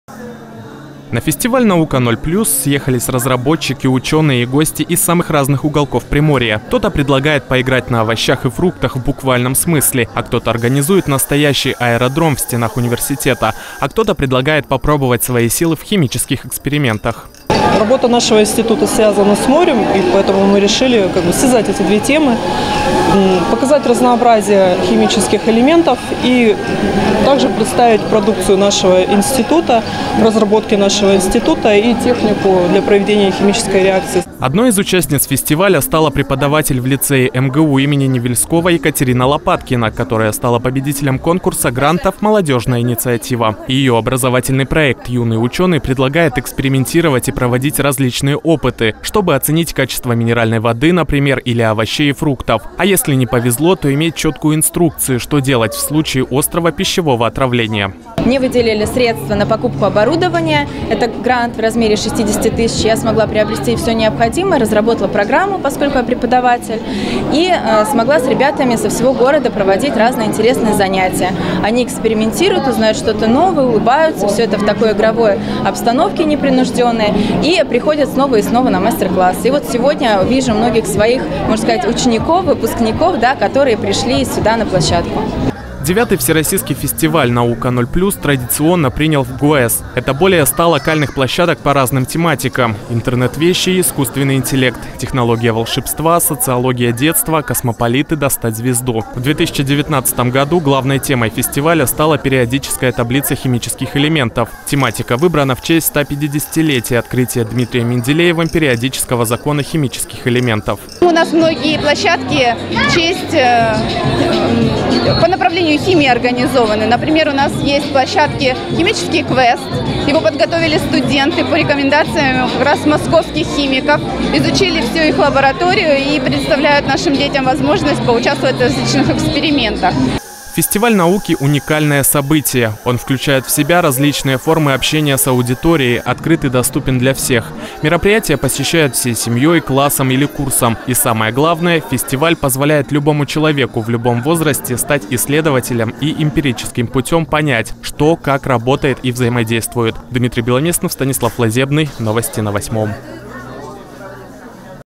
Фестиваль «Nauka 0+» прошел во Владивостоке
Проверить качество питьевой воды, пройти мастер-класс по каллиграфии, или изучить SMART-технологии в лаборатории «Умный город» смогли жители Владивостока. Во ВГУЭС прошел ежегодный фестиваль НАУКА 0+.